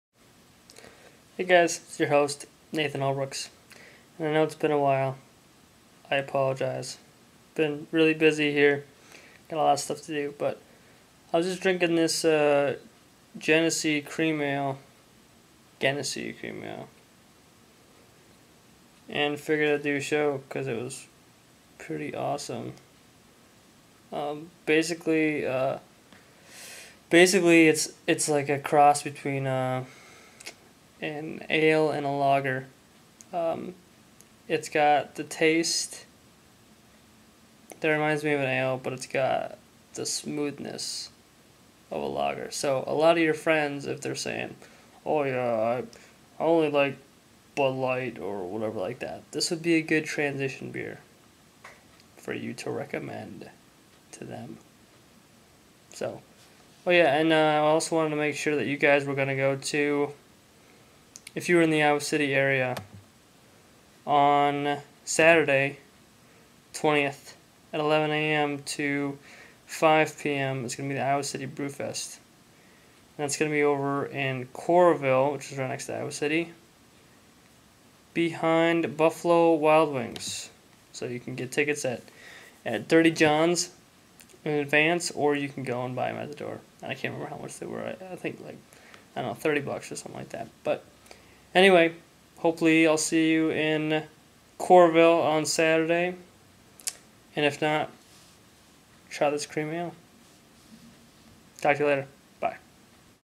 Genesee Cream Ale and other stuff Episode 11
Genesee Cream ale and talking about the upcoming beer event in Coralville, IA.